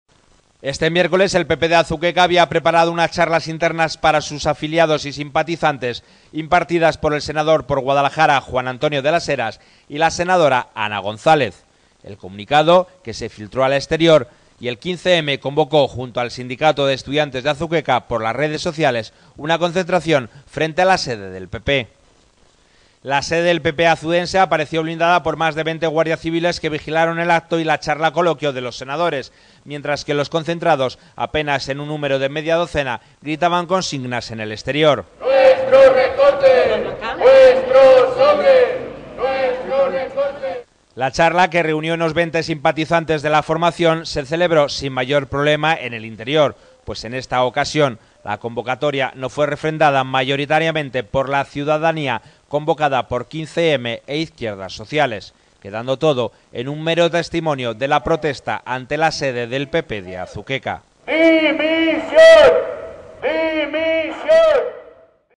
Protesta ante la sede del PP de Azuqueca
El miércoles el PP de Azuqueca había preparado unas charlas internas para sus afiliados y simpatizantes, impartidas por el senador por Guadalajara Juan Antonio de las Heras y la senadora y Ana González, el comunicado se filtró al exterior y el sindicato de estudiantes de Azuqueca convocó junto al 15M por las redes sociales una concentración frente a la sede del PP.
La sede del  PP azudense apareció blindado por más de 20 guardias civiles que vigilaron el acto y la charla coloquio  de los senadores, mientras que los concentrados apenas en número de media docena gritaban consignas en el exterior.
La charla, que reunión unos 20 simpatizantes de la formación, se celebró sin mayor problema en el interior pues en esta ocasión la convocatoria no fue refrendada mayoritariamente por la ciudadanía convocada por 15m e izquierdas sociales quedando todo en un mero testimonio de la protesta ante la sede del PP. de Azuqueca